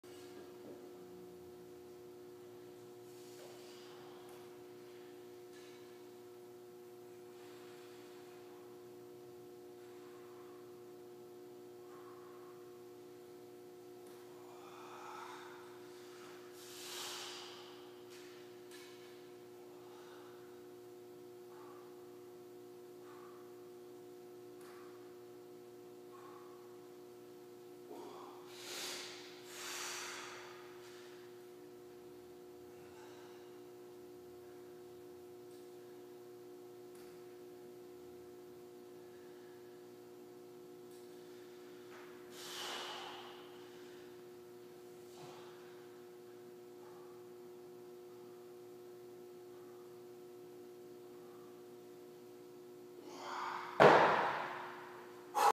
Equalizer Knee Ups to Vertical Press Hold
Proud of this, as I have a rotator cuff tear in the right, and doc says severe tendinitis in the left! Been working on stabilizing the shoulder girdle with alot of bodyweight work, and with the Lebert Fitness equipment!